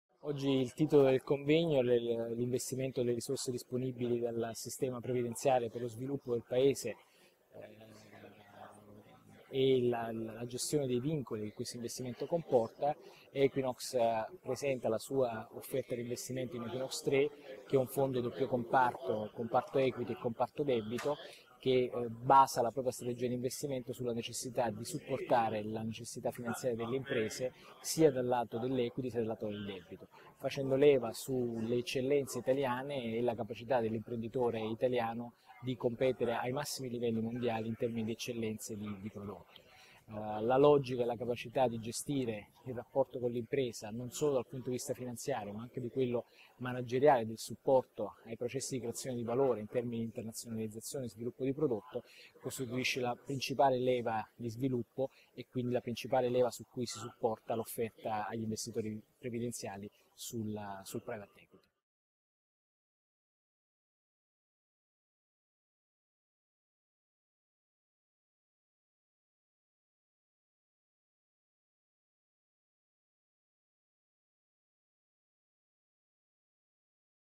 Giorgio Mercogliano - Equinox - Una piattaforma Equity e Debito per investire in Italia
Il punto di vista del Dott. Giorgio Mercogliano, Partner Equinox, sul tema del Convegno di Primavera 2016 "Patrimoni previdenziali: gli investimenti delle risorse tra vincoli di risultato e sviluppo del Paese". Come investire in Italia attraverso una piattaforma Equity e Debito?